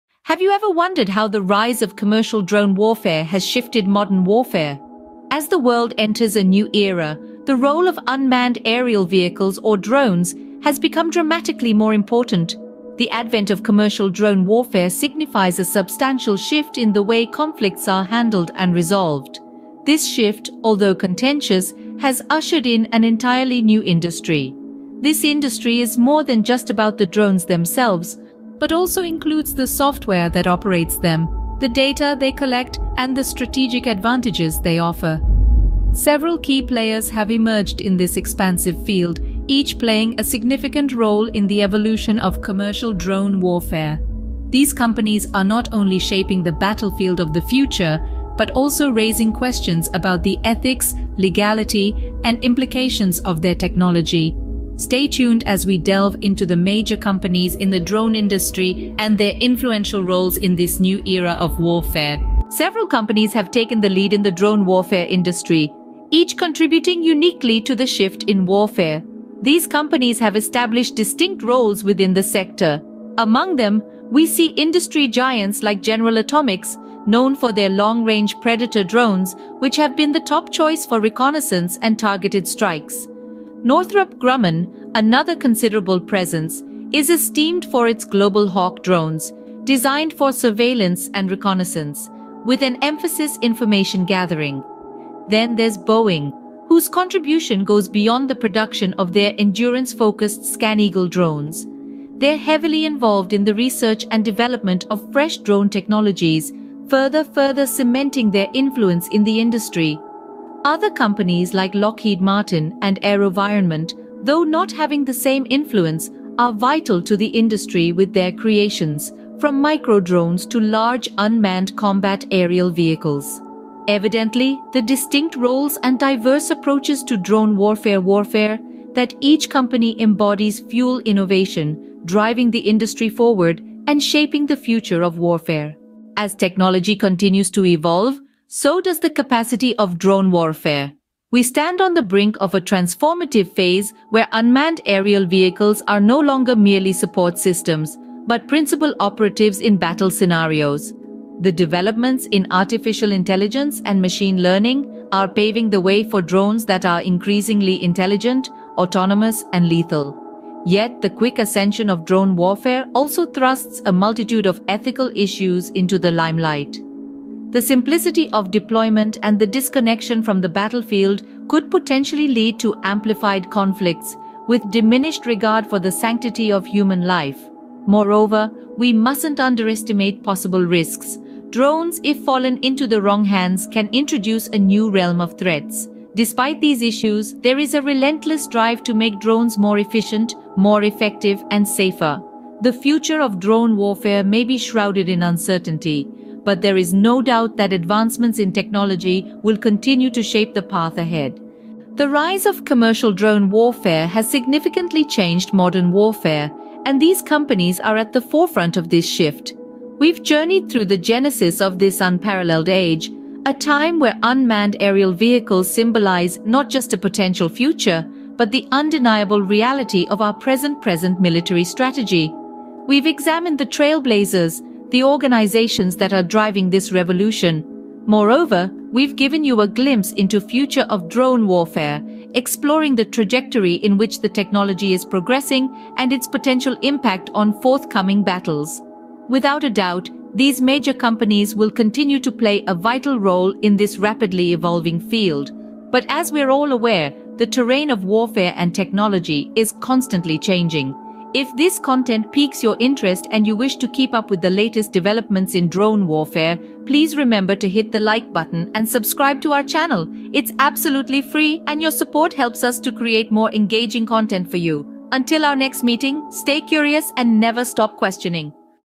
Drone Warfare
In this eye-opening video, we delve deep into the controversial world of drone warfare. Join us as we analyze available data and conduct thorough market research to uncover the commercial side of this industry. From defense contractors to technology pioneers, we shed light on the hidden players driving the drone revolution. Prepare to be amazed and shocked. Let's educate ourselves together!